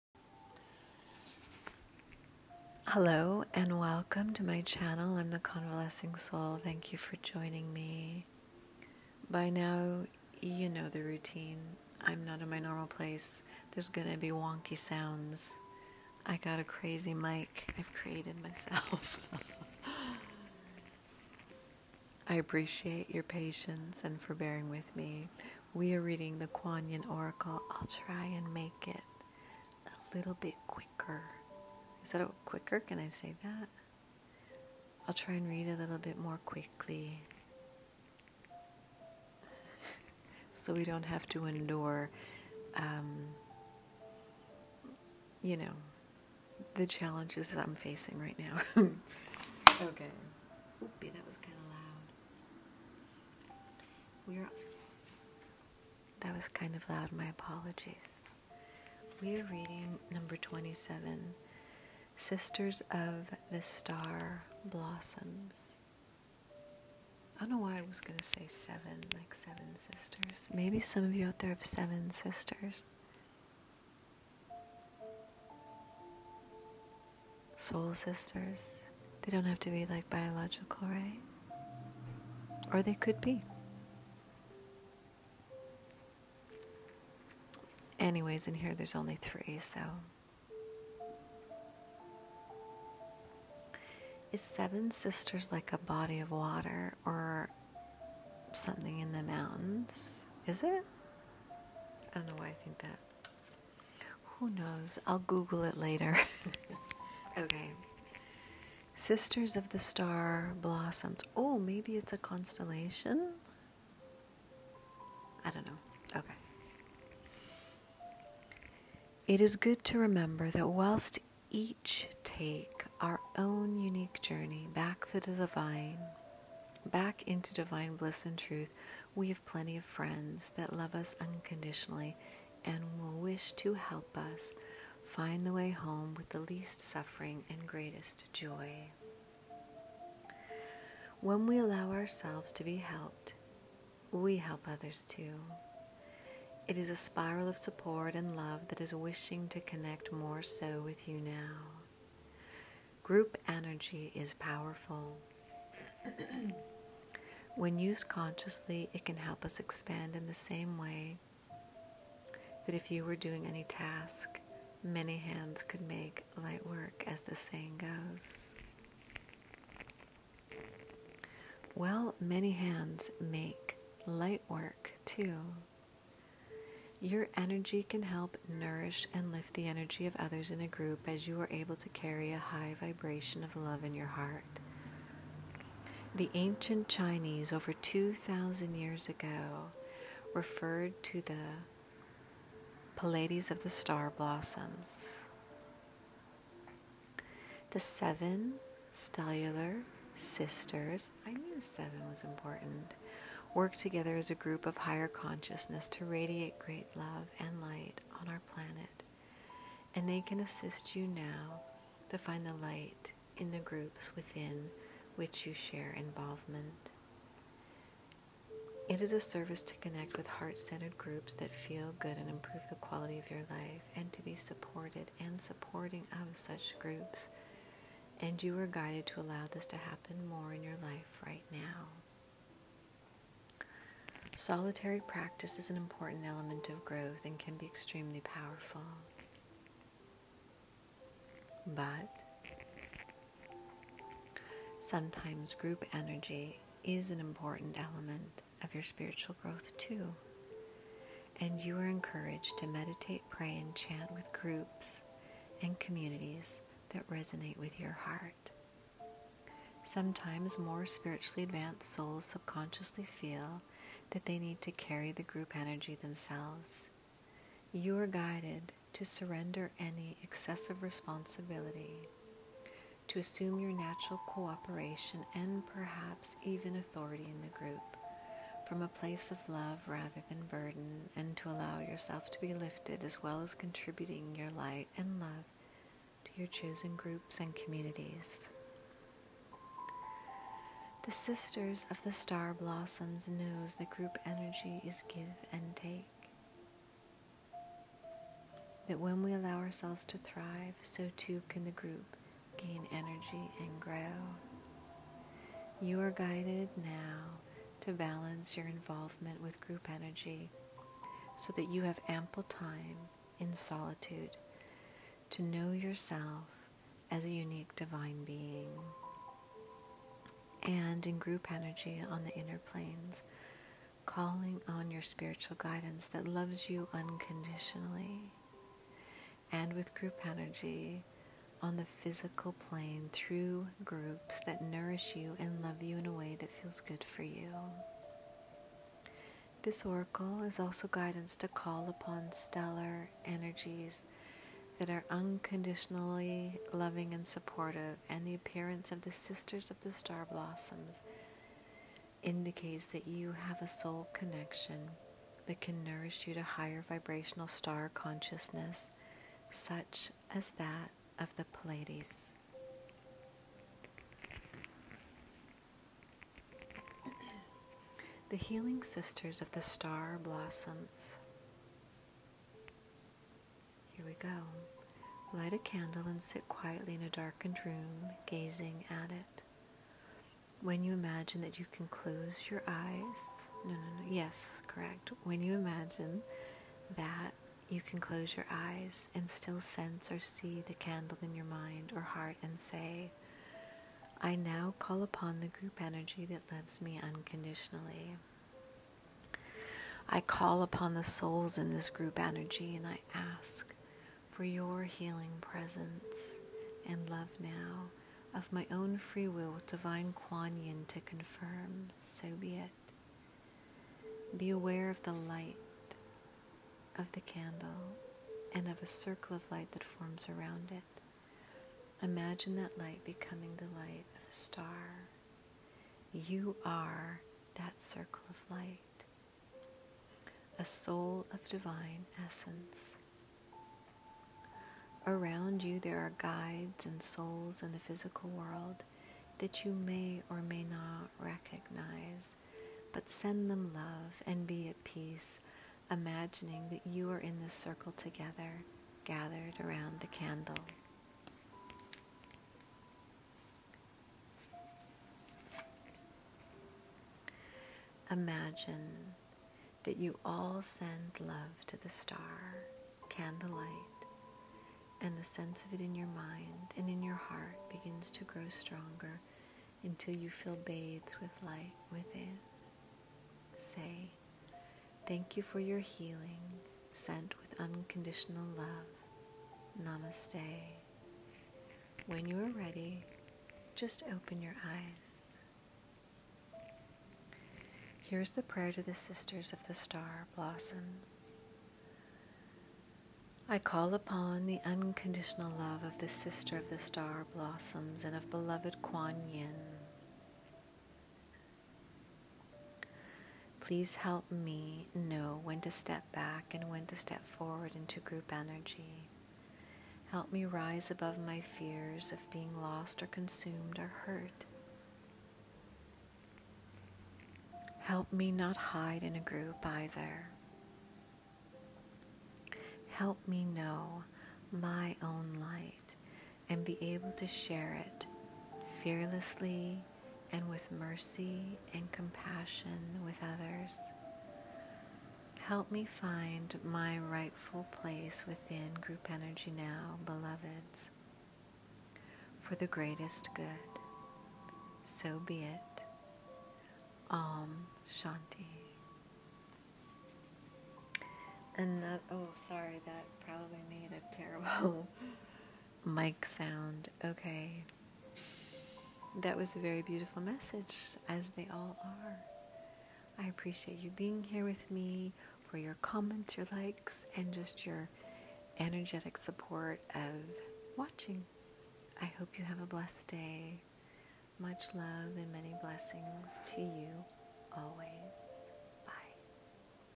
KUAN YIN ORACLE #27 - SISTERS OF THE STAR BLOSSOMS
KUAN YIN IS THE MOTHER OF COMPASSION.  THIS IS A READING IN A SERIES FROM THE KUAN YIN ORACLE GUIDEBOOK - BLESSINGS, GUIDANCE & ENLIGHTENMENT FROM THE DIVINE FEMININE.  ORACLES BY THE LOVELY ALANA FAIRCHILD AND ARTWORK BY ZENG HAO. HOPE THE GUIDANCE YOU FIND HERE SPEAKS TO YOU AND AIDS YOU AS YOU CONVALESCE BODY, MIND AND SOUL XOXO